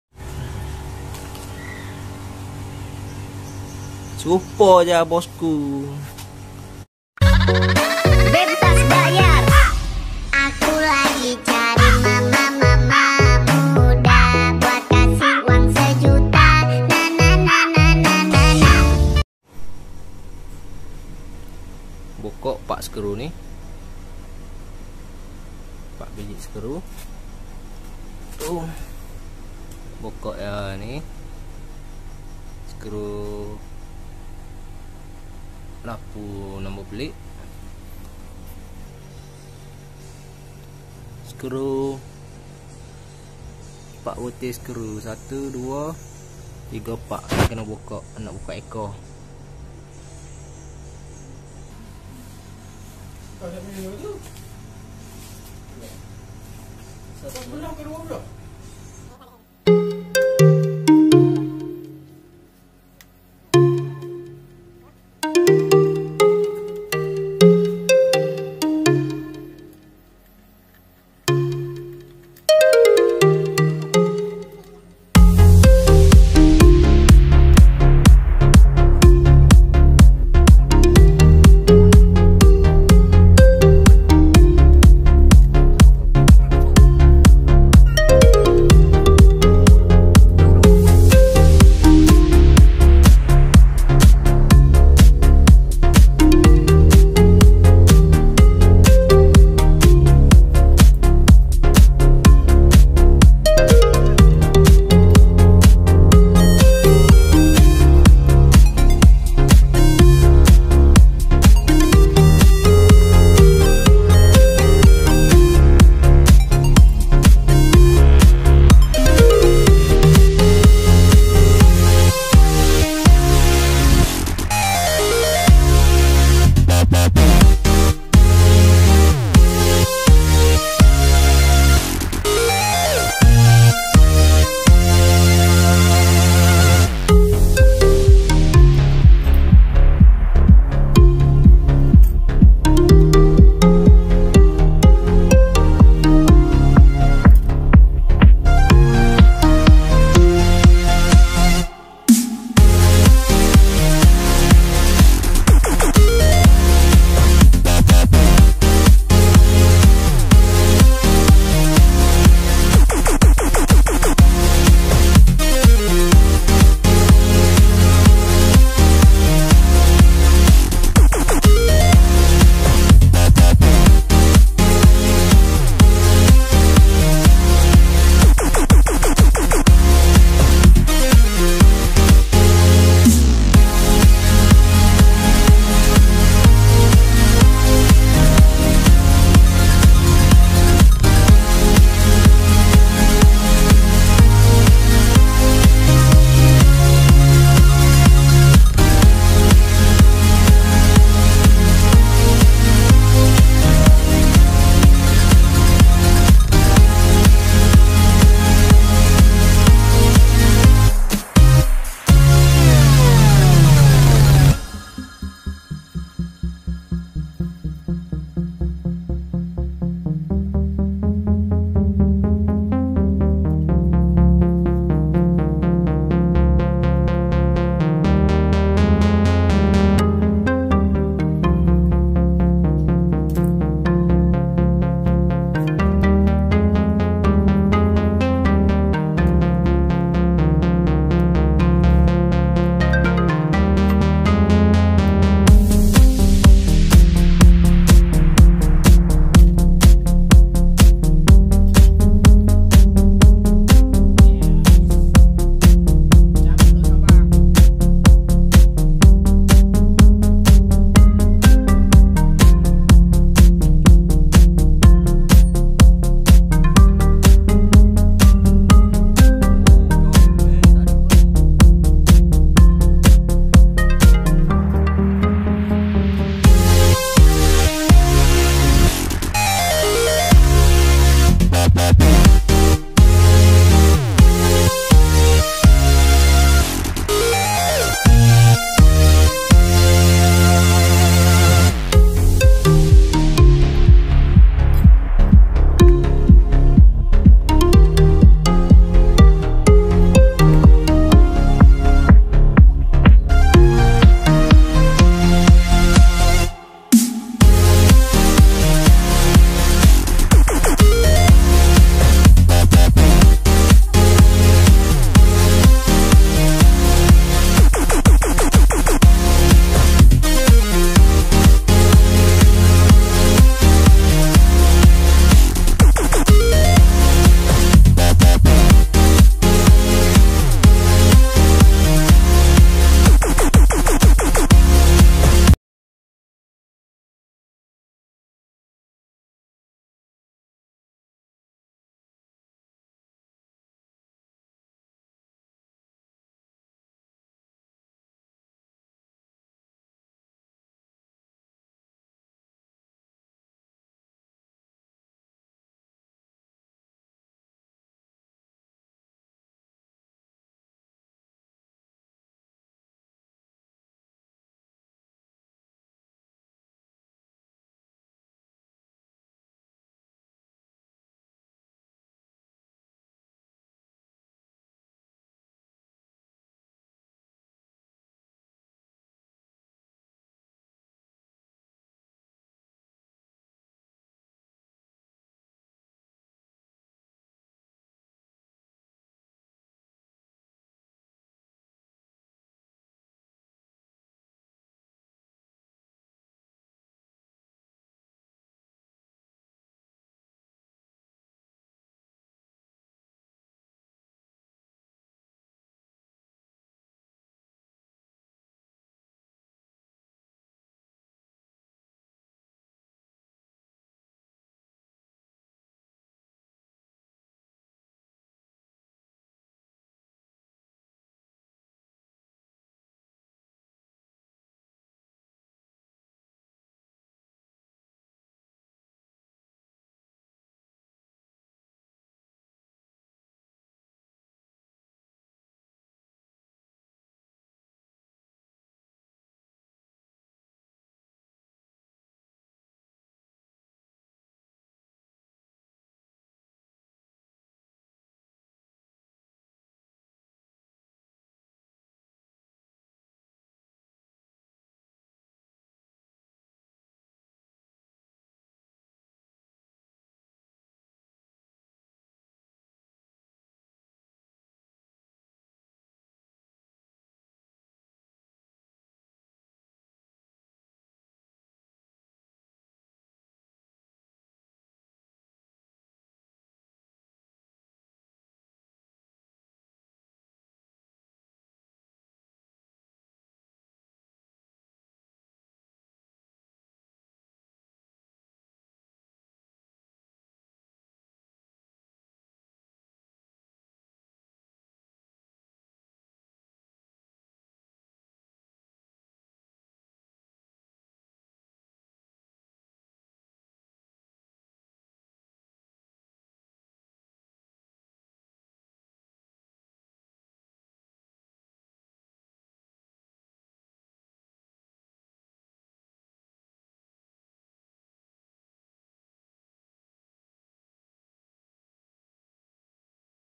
TUKAR EKOR Y15 ZR AKU | VIETNAM
Cara tukar ekor versi Vietnam y15 cyan/hijau petronas.